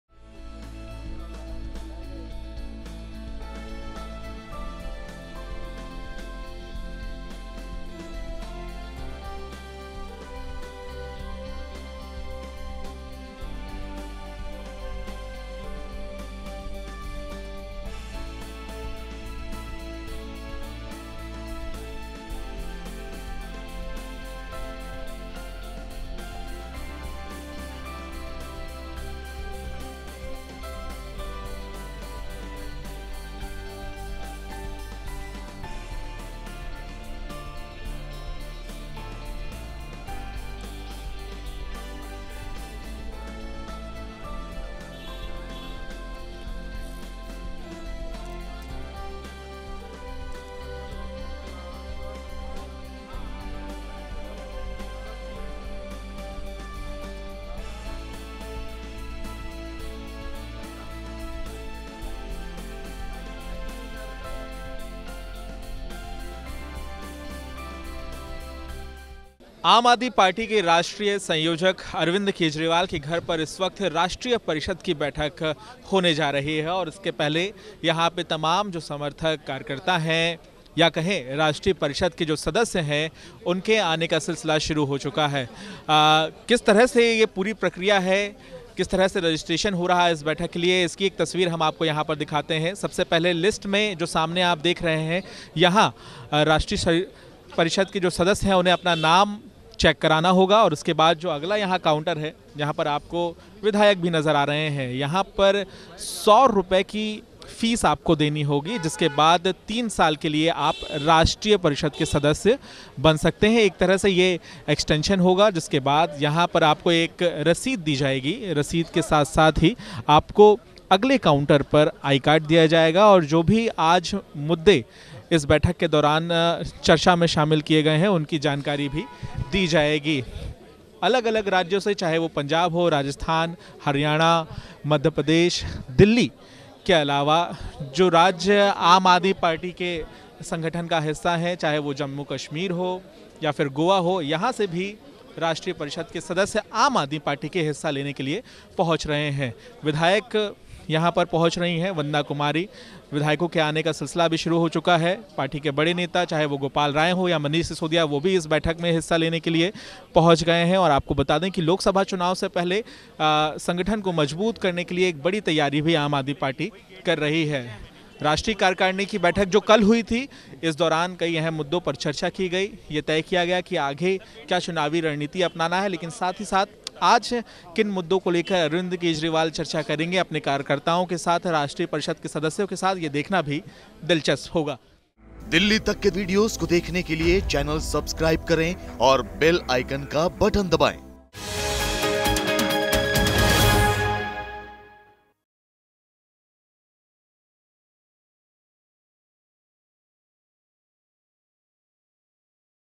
BREAKING: AAP के NATIONAL COUNCIL की मीटिंग शुरु,संबोधित करेंगे KEJRIWAL... | Dilli Tak
लोकसभा चुनाव की तैयारी के लिए आम आदमी पार्टी ने शनिवार को मुख्यमंत्री आवास पर राष्ट्रीय परिषद का बैठक बुलाया है. शुक्रवार को तय हुए एजेंडा पर आज राष्ट्रीय परिषद में चर्चा होगी.